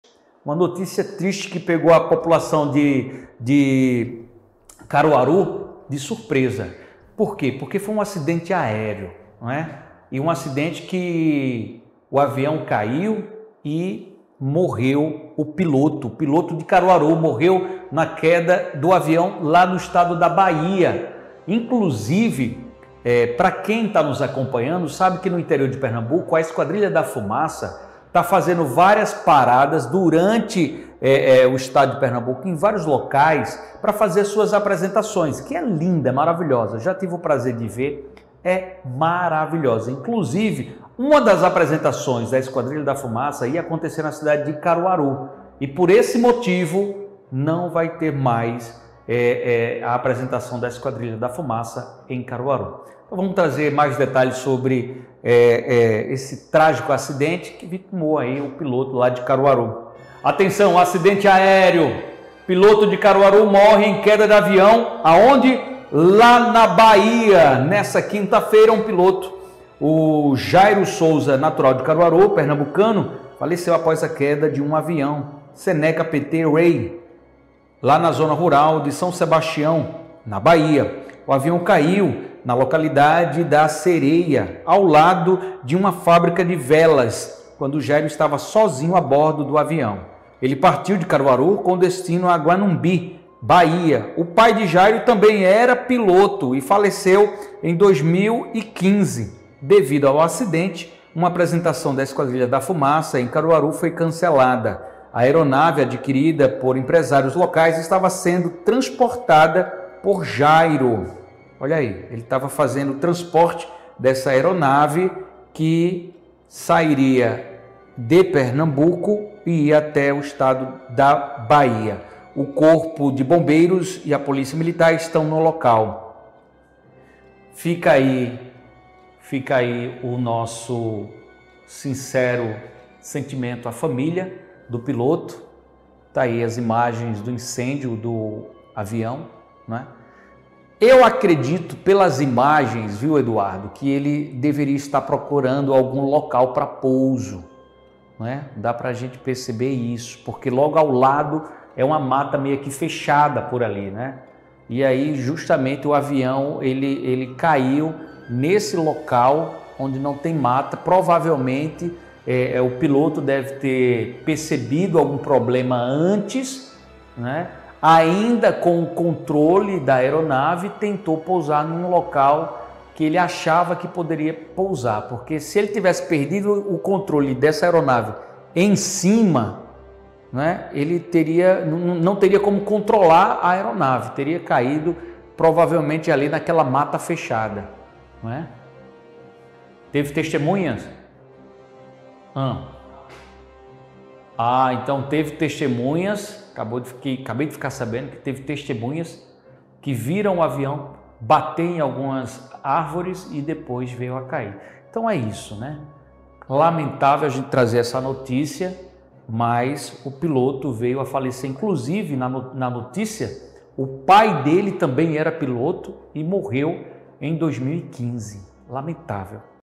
ACIDENTE AÉREO - Piloto de Caruaru morre em queda de avião na Bahia
ACIDENTE AÉREO
Piloto de Caruaru morre em queda de avião na Bahia